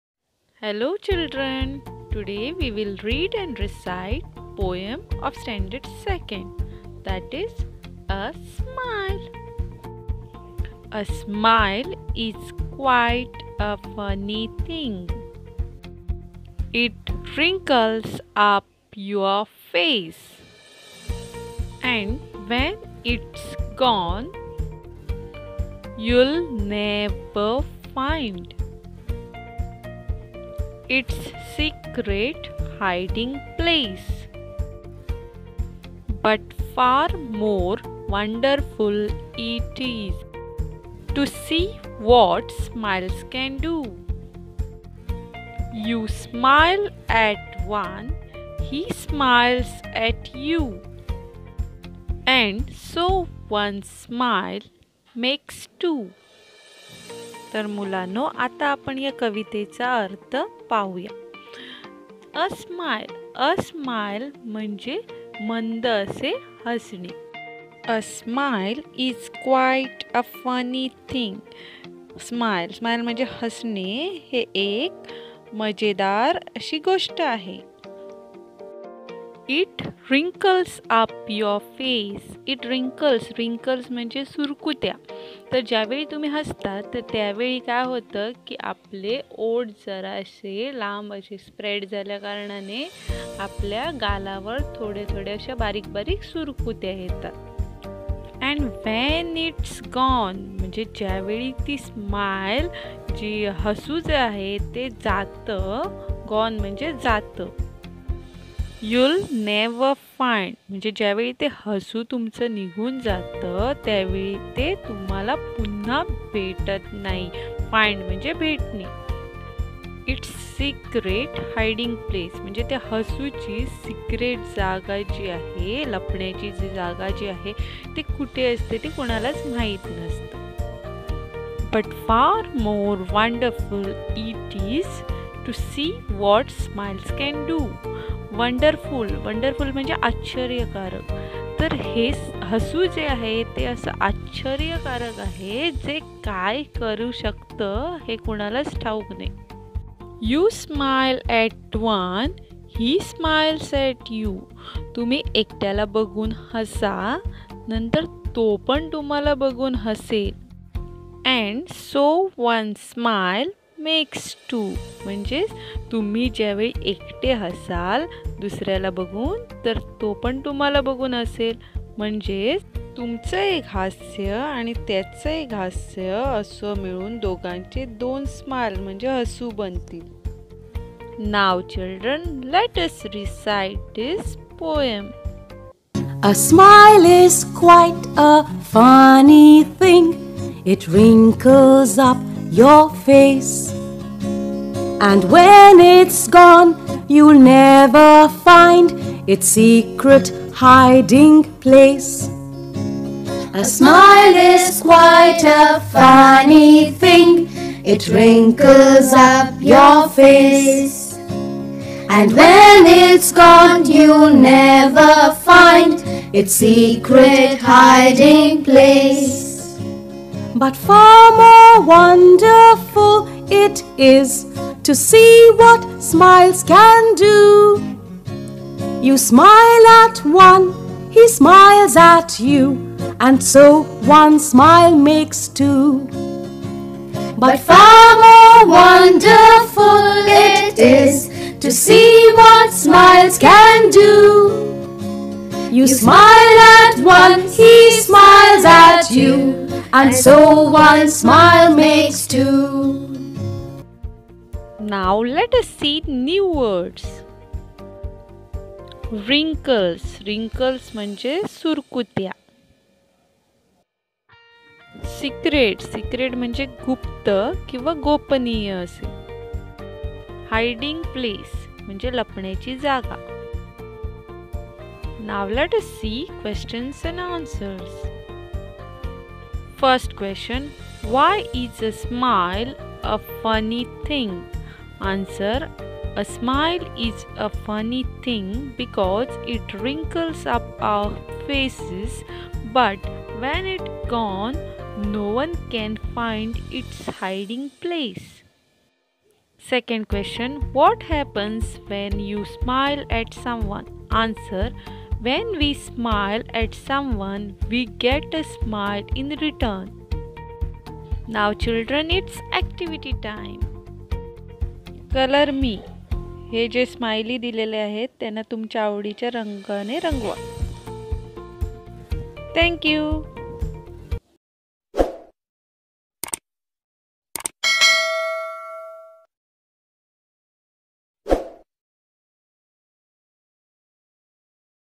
A SMILE😊(poem)# Std 2# with Marathi explanation # Questions &Answers New words meaning
A Smile poem with full marathi explanation  with pictures
Learn questions and answers
New words with marathi  explanation  and pictures
Do Activity and enjoy poem

My channel
Learning  with Ratika Chavan